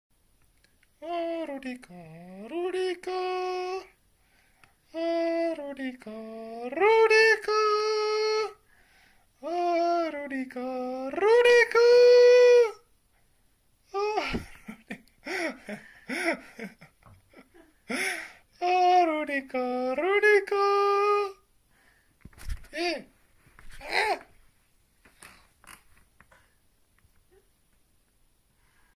Tribute to Mr. Kitty
This is a Kitty Song as tribute to my cat..